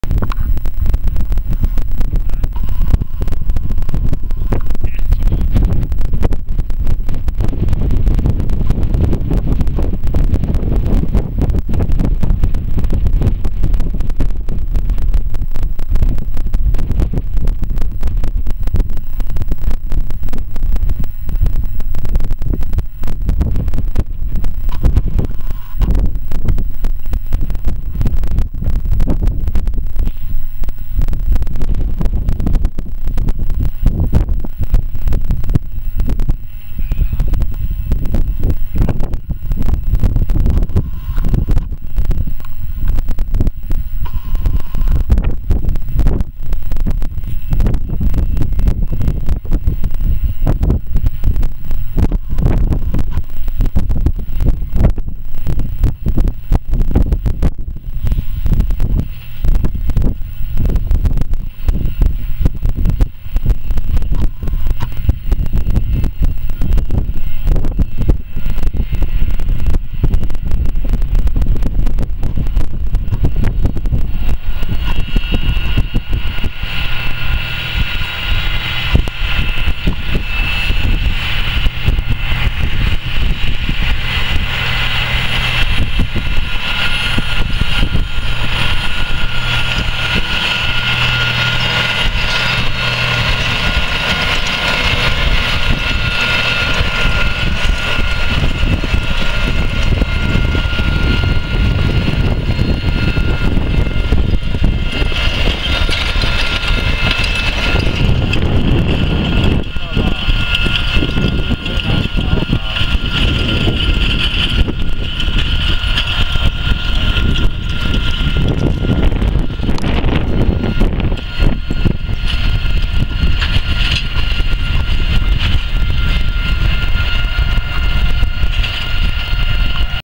John Deere 8310R tractor planting corn in Saskatchewan
John Deere 8310R tractor with Case IH 1250 Early riser 24 row  planter, planting corn in Saskatchewan. Still cold and very windy but going a good job!! The corn is for silage.